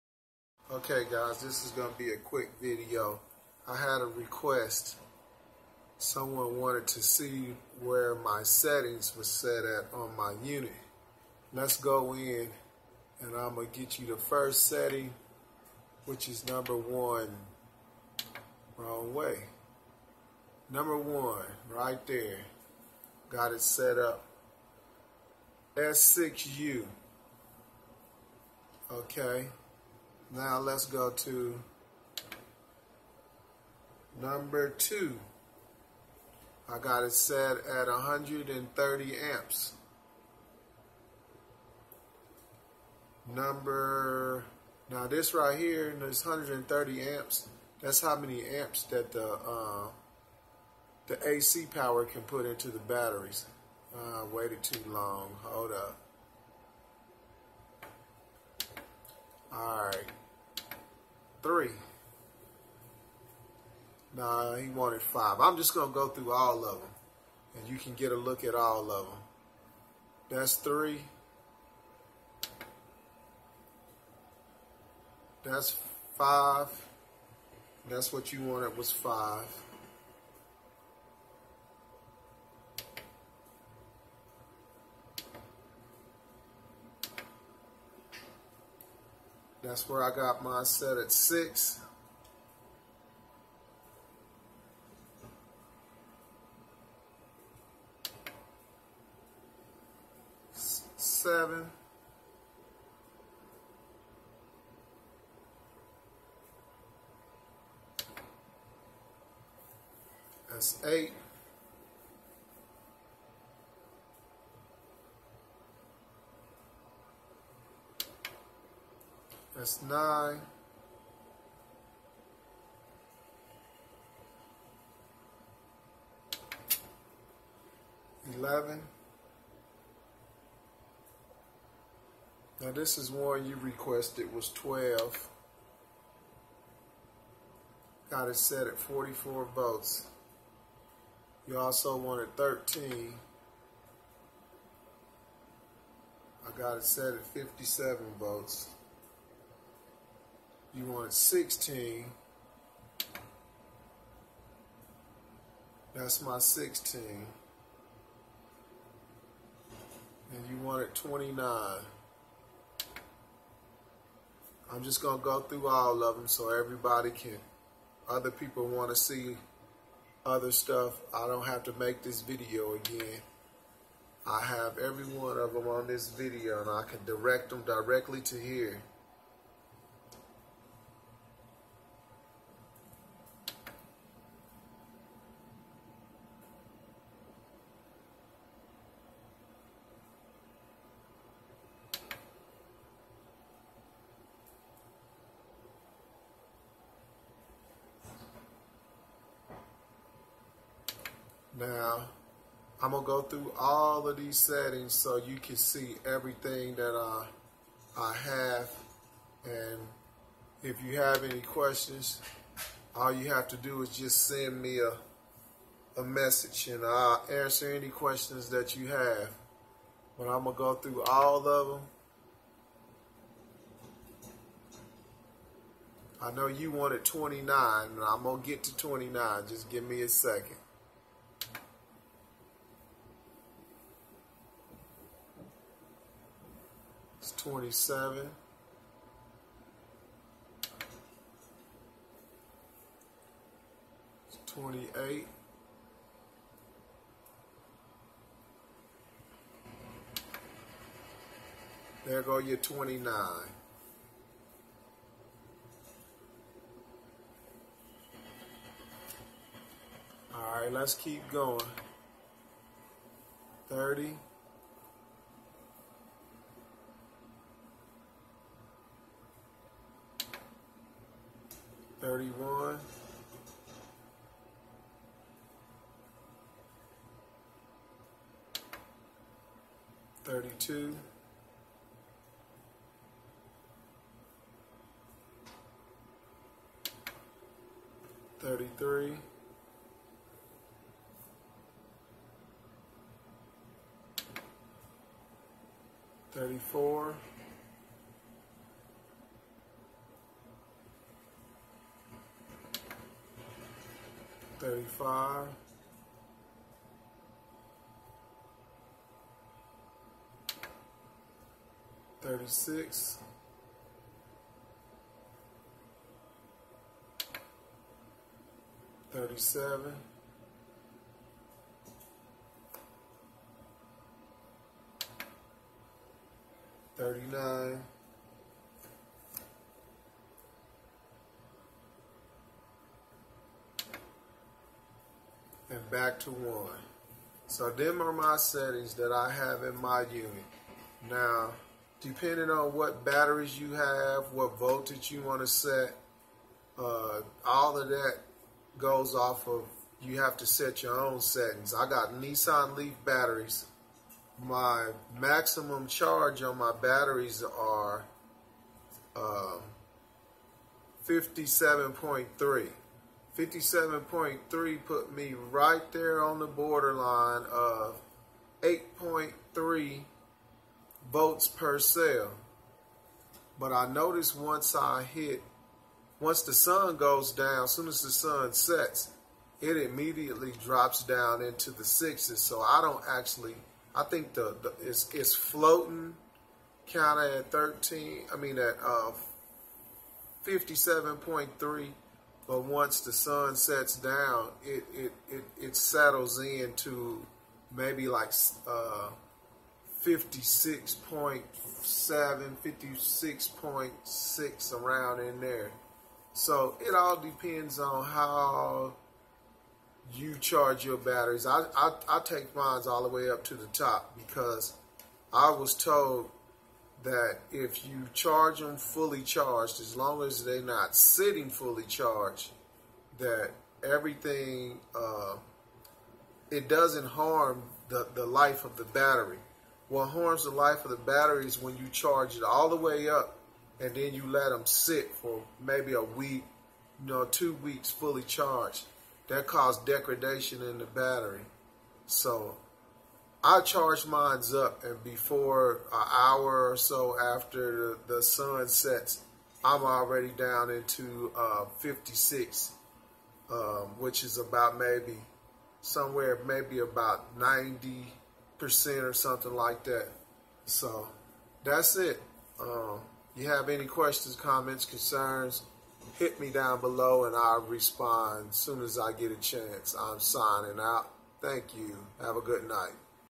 My settings on my MPP 5048 inverter.
I had someone asking to see the settings on my inverter.